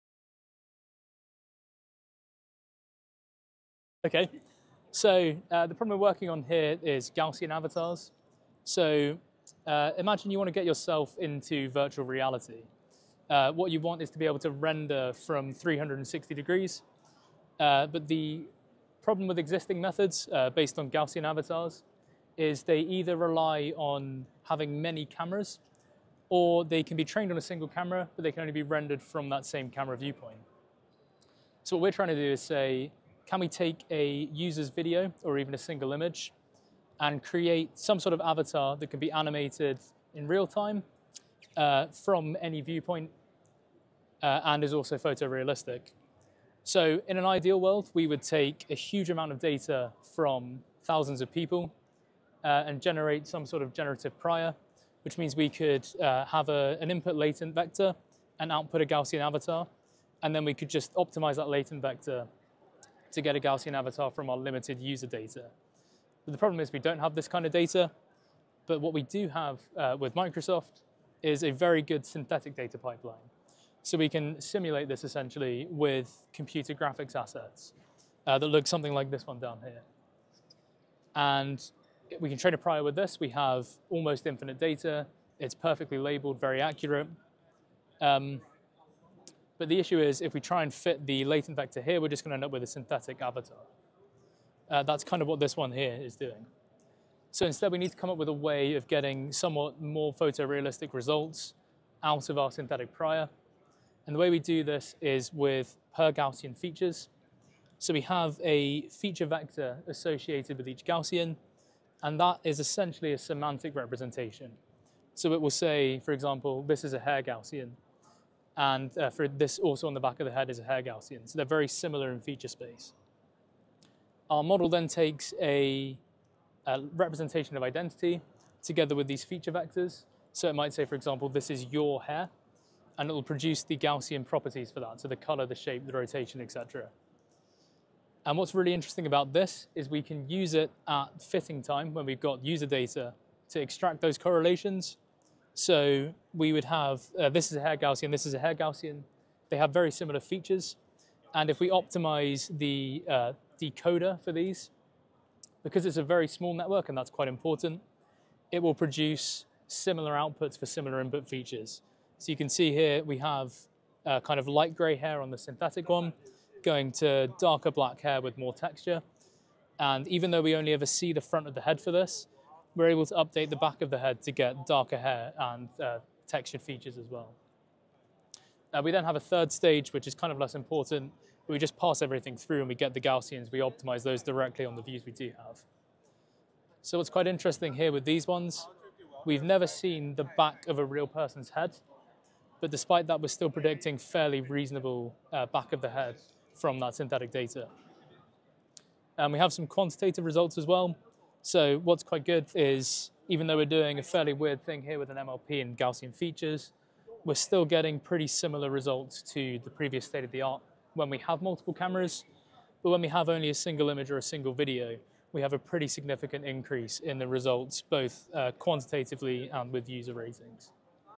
CVPR 2025: GASP: Gaussian Avatars with Synthetic Priors
GASP (Gaussian Avatars with Synthetic Priors) is a newly introduced framework for generating photorealistic, real-time, 360° animatable human avatars using only a single image or short monocular video. Unlike prior approaches that require complex multi-camera rigs or result in poor quality from novel views, GASP addresses this challenge by training a generative prior on synthetic data with perfect ground truth. This prior enables the model to infer unseen viewpoints—like the back of the head—during avatar creation, even when enrolled with minimal input data. The framework attaches 3D Gaussians to mesh triangles and learns per-Gaussian semantic features, which are then fine-tuned to match real user data through a three-stage fitting pipeline.

Empirical results on the NeRSemble dataset show that GASP outperforms several state-of-the-art baselines in both qualitative and quantitative metrics, including PSNR, SSIM, FID, LPIPS, and human-rated perceptual quality. The system achieves 70fps rendering on a consumer GPU and supports semantic latent space manipulations such as editing age, hairstyle, or facial hair. Ablation studies highlight the critical role of the synthetic prior, canonical Gaussian templates, and multi-stage optimization. Although GASP can occasionally produce synthetic-looking results in occluded regions, it represents a strong step toward scalable, real-time avatar creation using commodity hardware, with no neural networks needed at inference time.